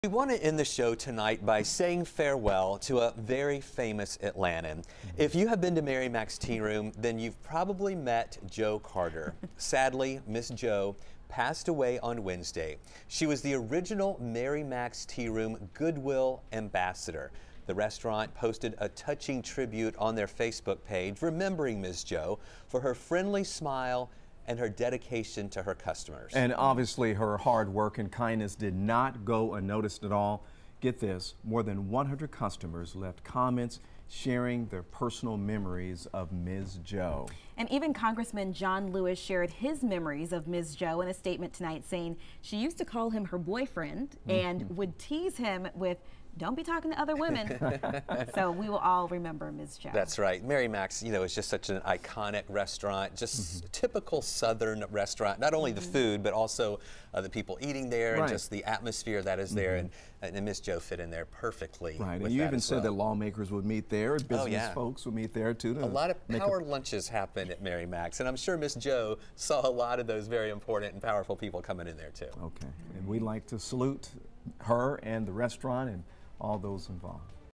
Mary Macs Tearoom's 'Goodwill Ambassador' dies
Jo Carter was a legend at Mary Macs Tea Room for 25 years.